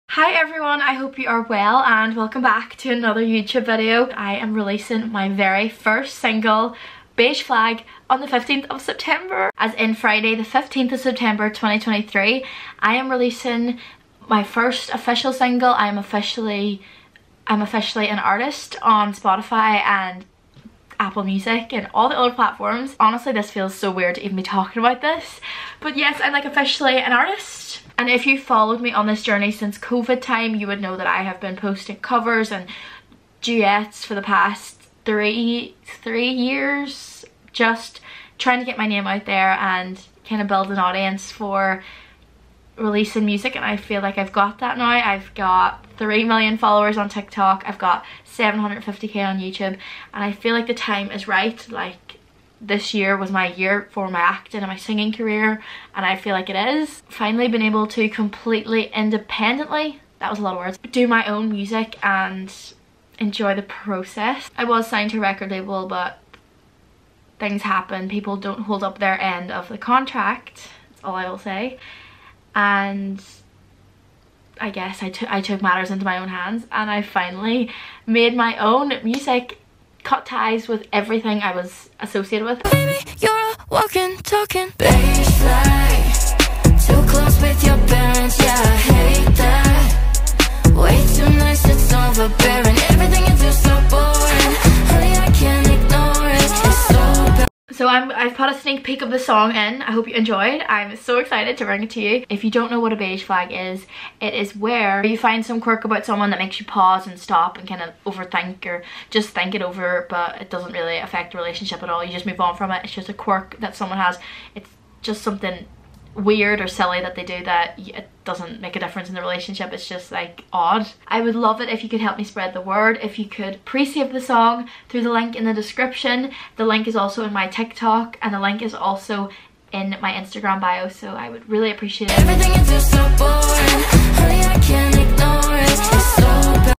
MY FIRST SINGLE! OUT THIS FRIDAY 15TH SEPTEMBER! ANNOUNCEMENT!
4 DAYS AWAY FROM RELEASE! OUT SEPTEMBER 15th!! SO EXCITED